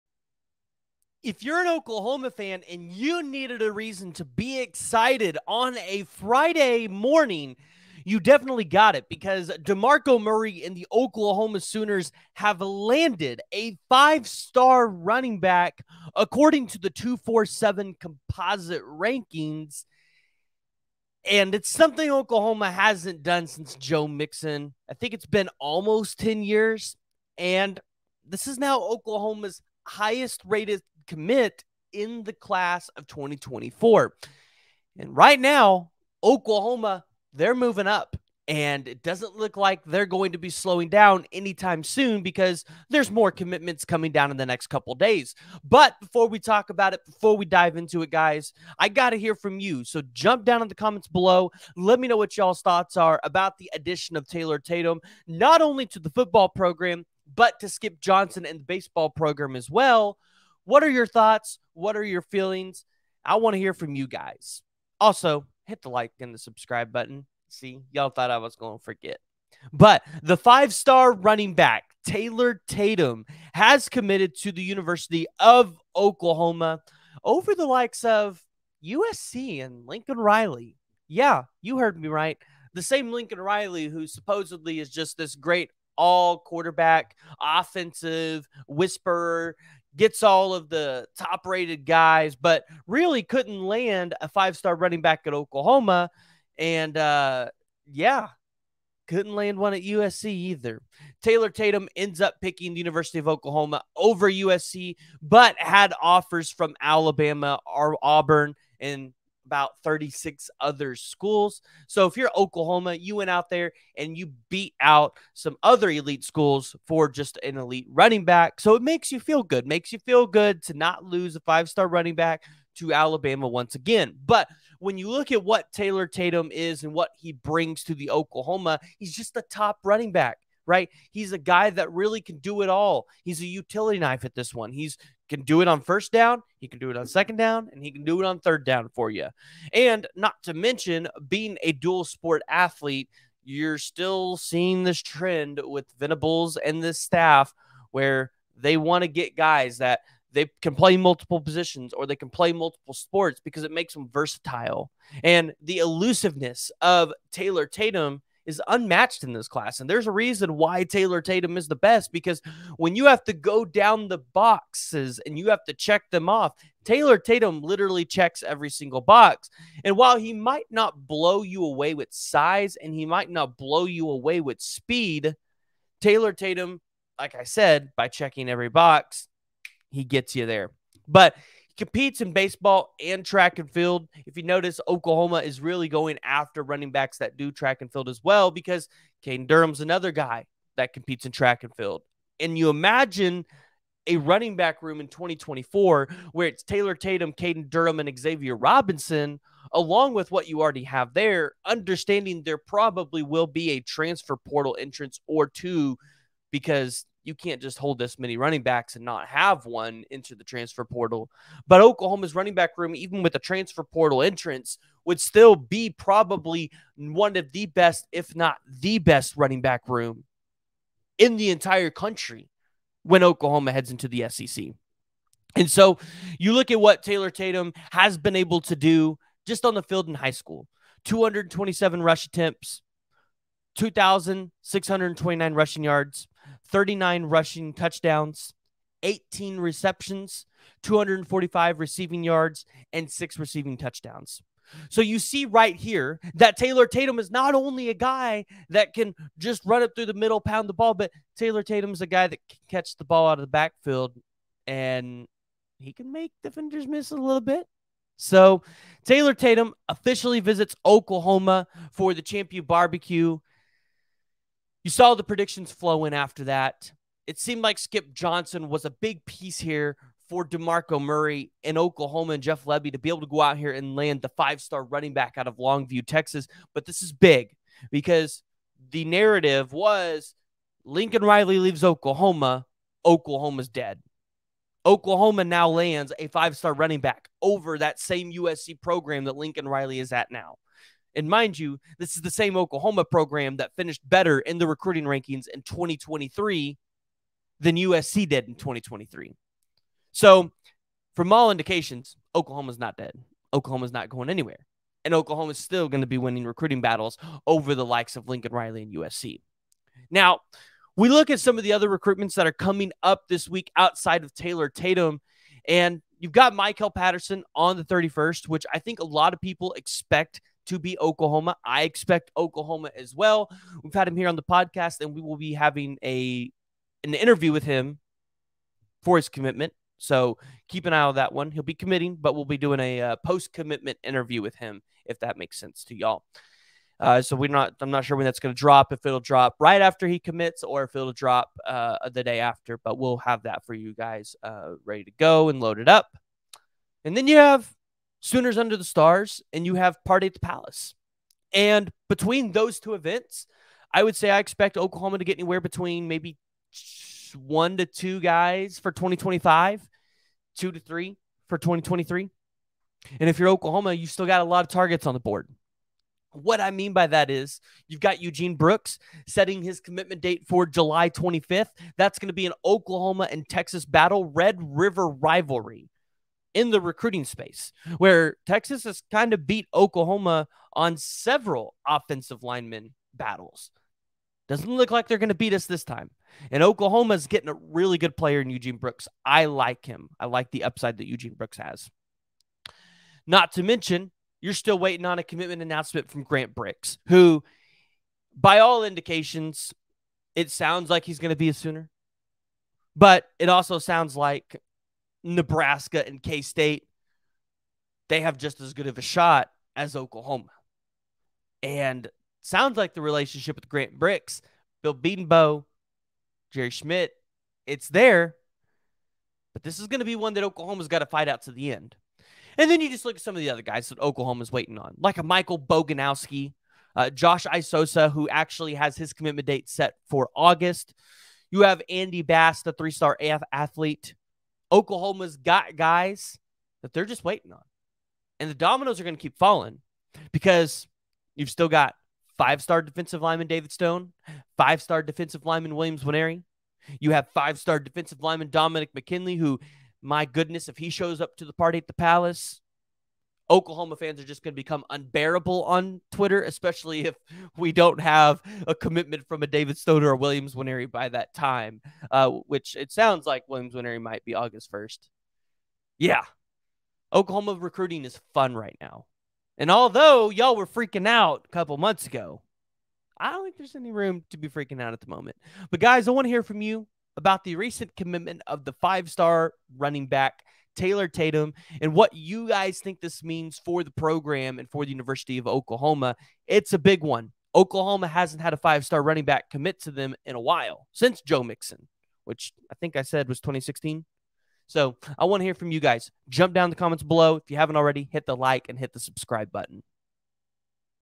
OU Football SECURE 5 ⭐️ Dual Sport Dynamo | Oklahoma Sooners GET Taylor Tatum | Demarco Murray
The Oklahoma Sooners have landed a MASSIVE 5 star RB out of longview TX and now the Oklahoma Sooners are positioning themselves to potentially land one of the best classes in OU Football history. The Oklahoma Sooners seemingly are rolling after the ChampU BBQ, which was their biggest official visit recruiting weekend of the year but this time they were able to get help from Skip Johnson and the OU Baseball program!